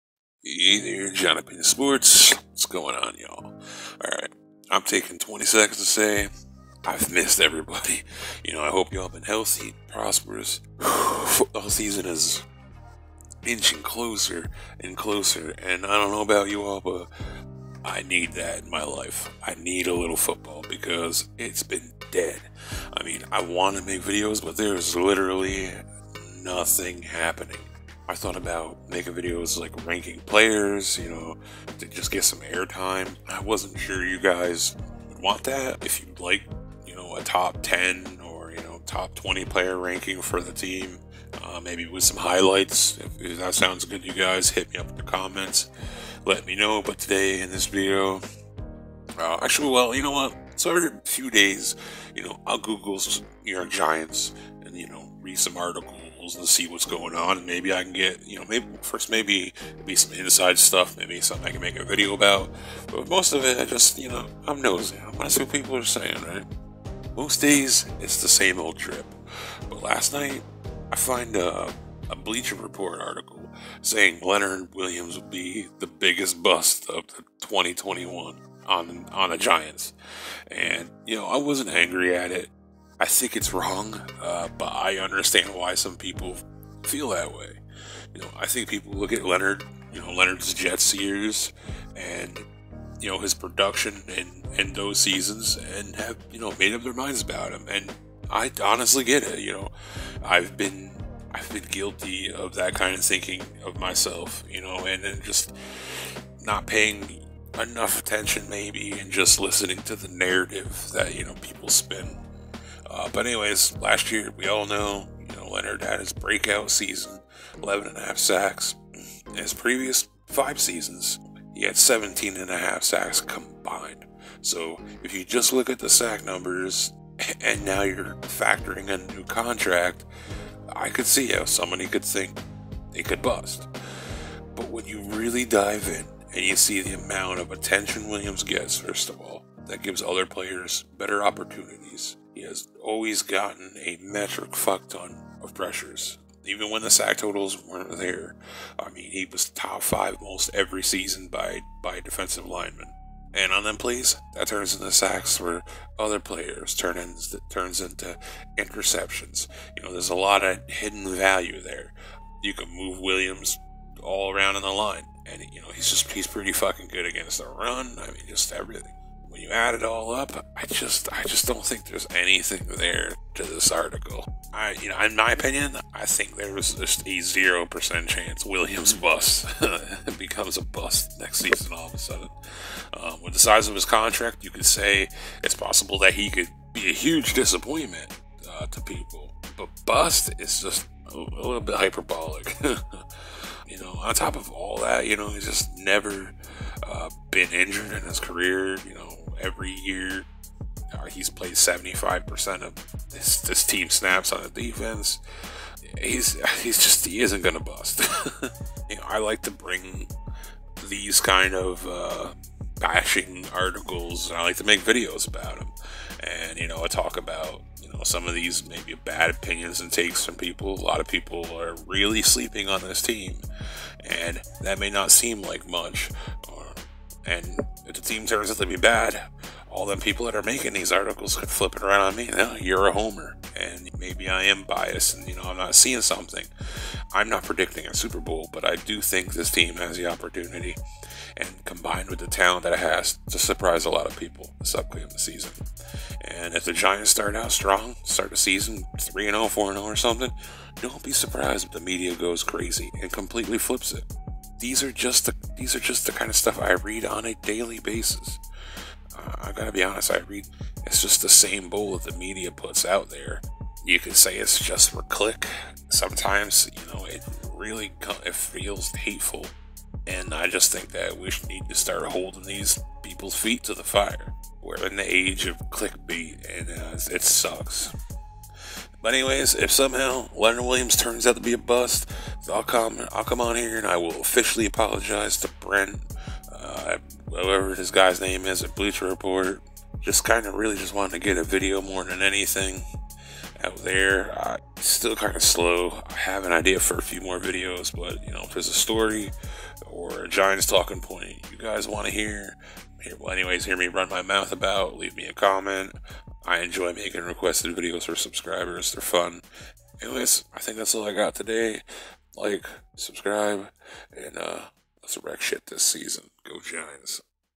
New York Giants | Bleacher Report predicts Leonard Williams will be teams biggest bust in 2021
I respond to Bleacher Report's article saying Leonard Williams will be the biggest bust of 2021 on the Giants team.